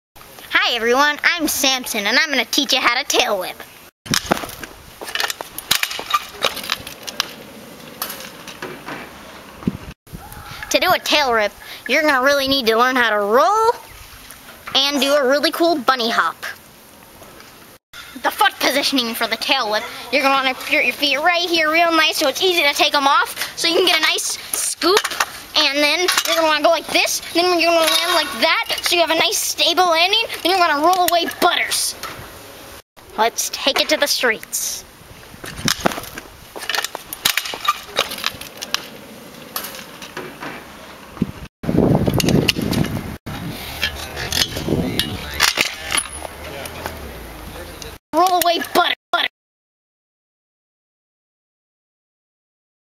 Samson Trick Tip Tutorial: Tail Whip
Samson Smith teaches you how to do a tail whip. If you want to see more Samson smith videos please leave a commment below.